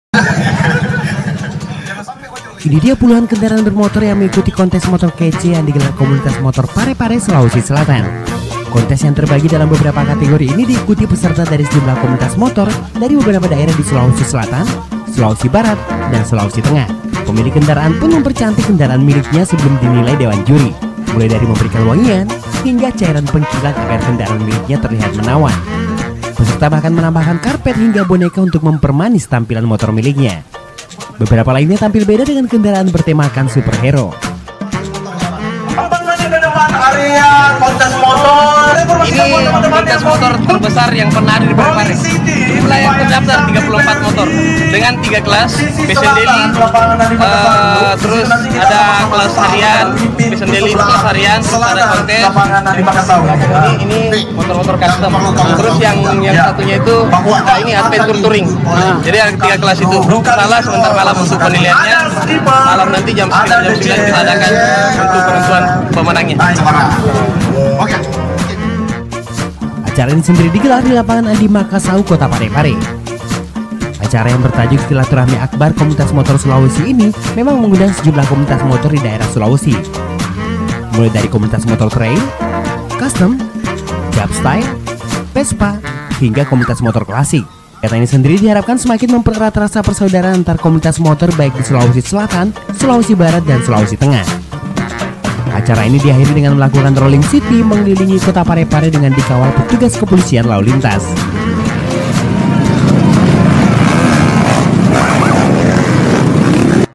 Keseruan konten Motor di parepare
Parepare memang punya banyak komunitas motor
jadi jangan heran jika di kota ini banyak komunitas motor yang terbentuk.